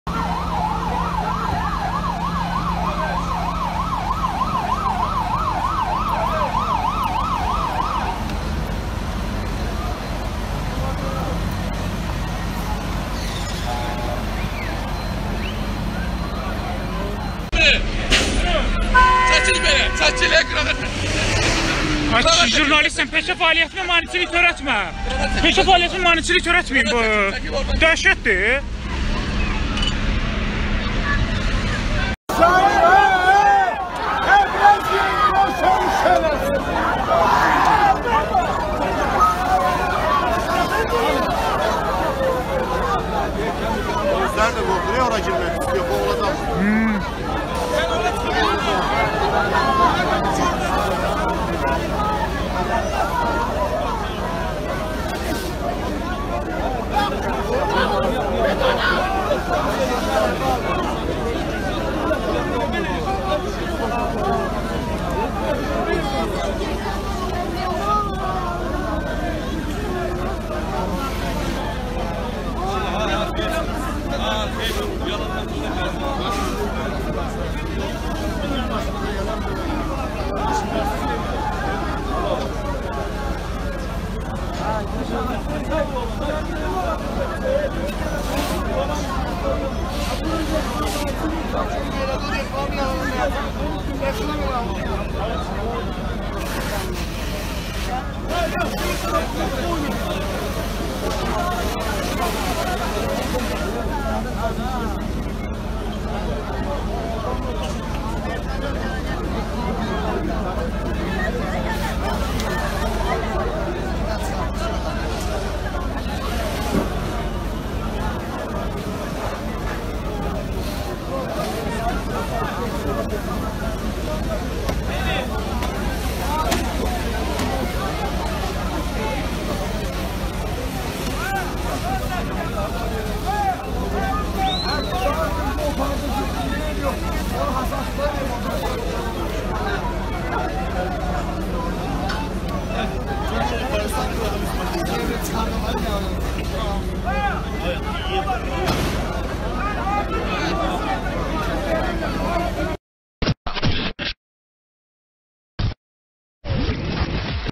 Neftçilərdə mağaza yanğınları başladı
Diqlasın yaxınlığındakı X-Lady və İzabel mağazalarında
yanğın baş verib. Video - Vəli Quliyev